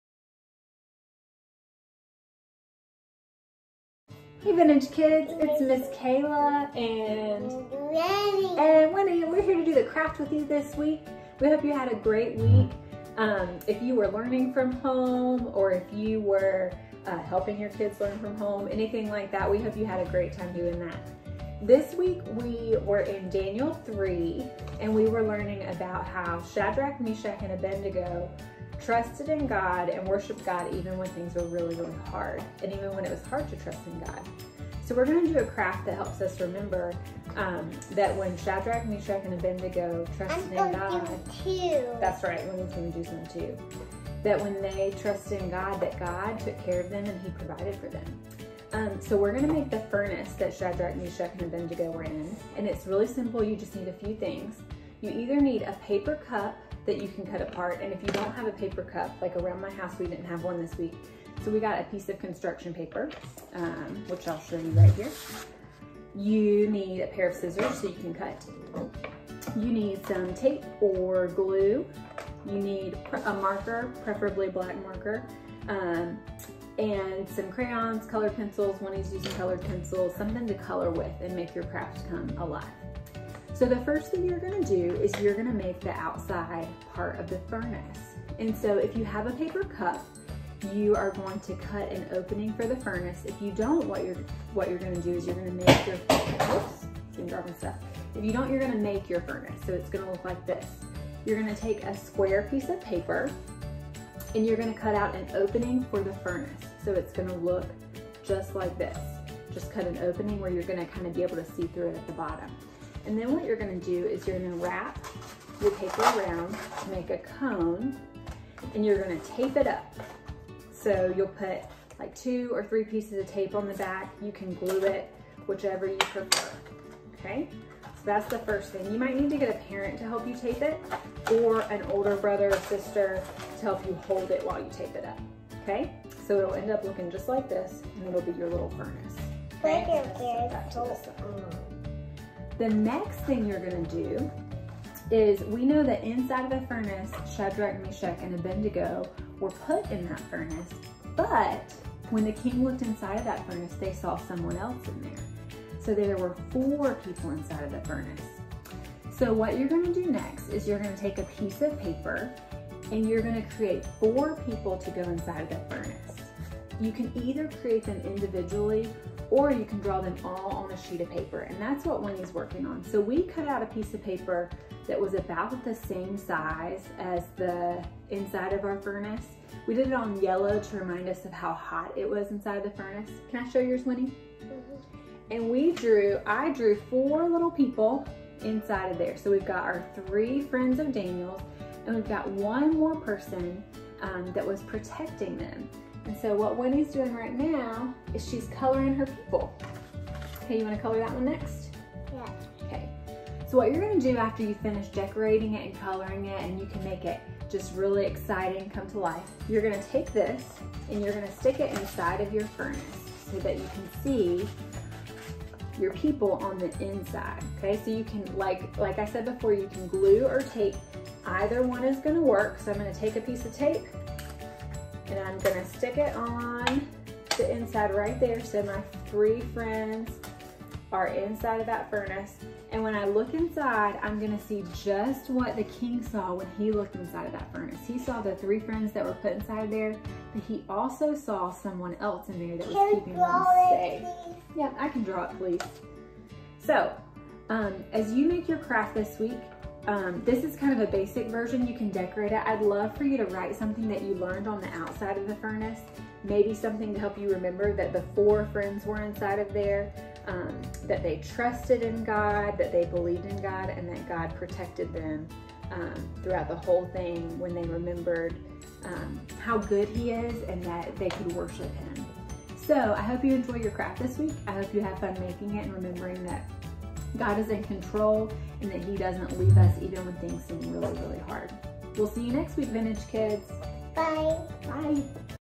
Vintage Kids Craft - Aug 30
Kids Craft for Aug 30 - Hey Vintage Kids! Watch the Pleasants Family  demonstrate the craft for this week's story with the “Four in the Furnace Craft”!  Have fun making your craft as a family!!